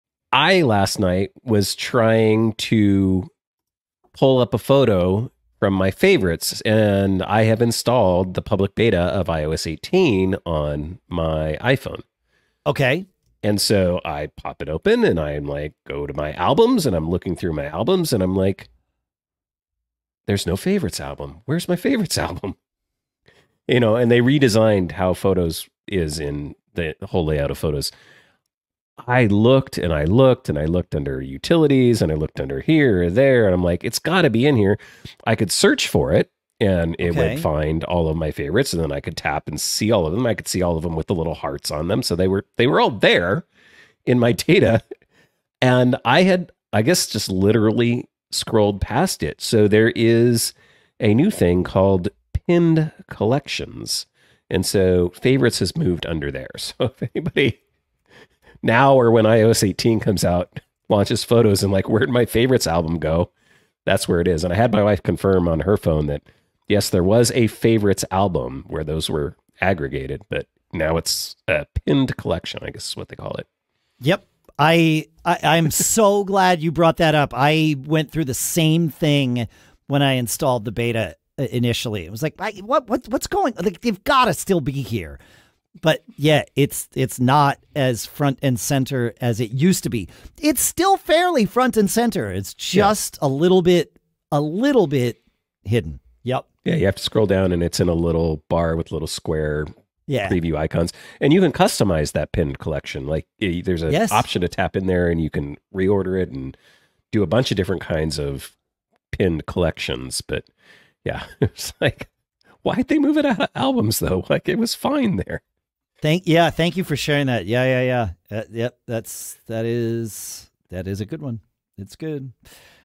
Where Did My Favorites Album Go in iOS 18? Pinned Collections Explained!
In this segment, we discuss how iOS 18 changes the way you access your Favorites album in the Photos app. If you’ve installed the public beta or plan to upgrade, you may wonder, "Where did my Favorites album go?" With the new Pinned Collections feature, your Favorites are no longer in the usual spot. Learn how to find and customize your Favorites in iOS 18, plus a few tips on navigating the redesigned Photos layout.

0:00 Trying to find my Favorites album in iOS 18
0:23 Exploring the new layout in Photos
0:49 Discovering the Pinned Collections feature
1:14 How to access and customize Pinned Collections in iOS 18

Don’t miss out on learning something new with each episode of Mac Geek Gab!